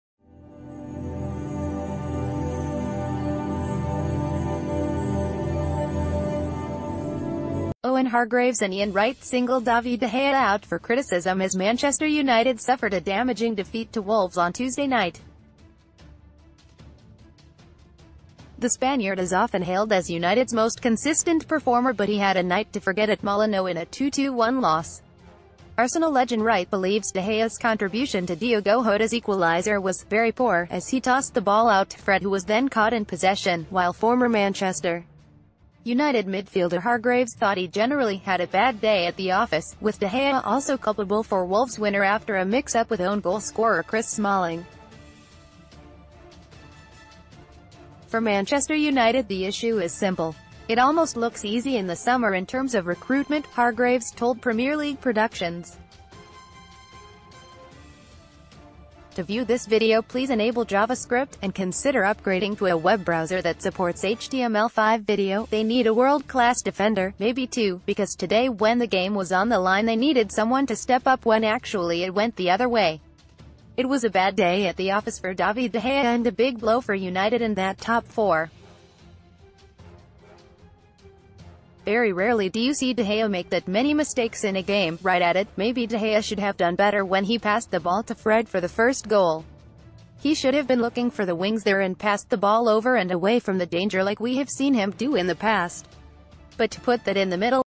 ManUtd News - 'Very poor' Manchester United star singled out after Wolves defeat
Owen Hargreaves and Ian Wright singled David de Gea out for criticism as Manchester United suffered a damaging defeat to Wolves on Tuesday night. The Spaniard is often hailed as United’s most consistent performer but he had a night to forget at Molineux in a 2-1 loss. Arsenal legend Wright believes De Gea’s contribution to Diogo Jota’s equaliser was ‘very poor’ as he tossed the ball out to Fred who was then caught in possession, while former Manchester United midfielder Hargreaves thought he gen...